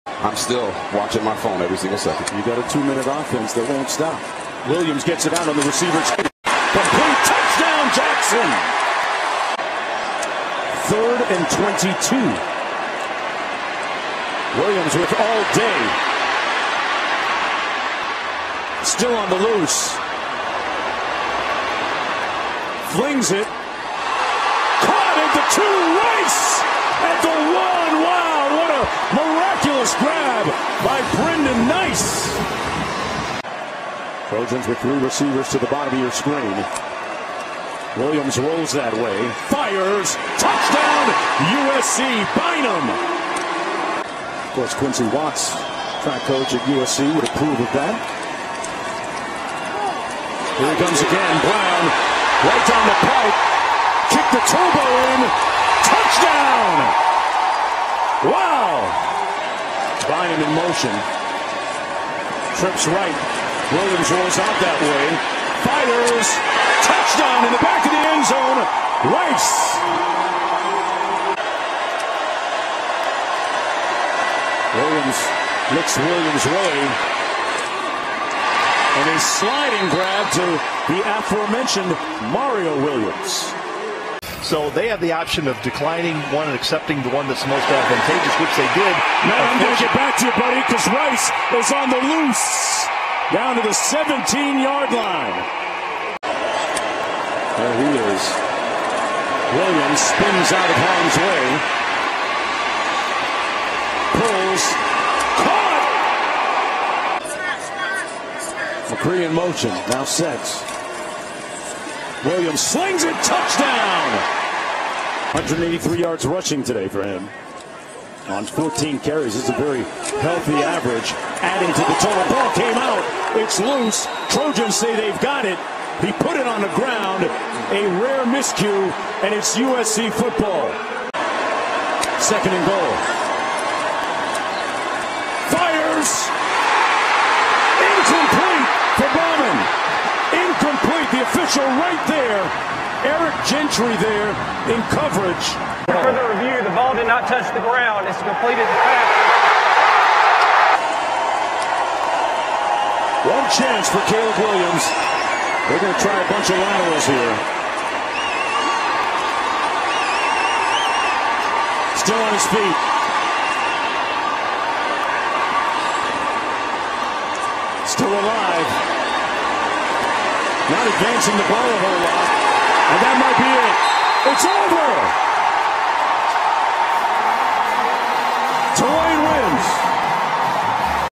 Football - Tulane 46, USC 45: Cotton Bowl Highlights (1/2/2023)
The Trojans fall to the Green Waves in the Cotton Bowl.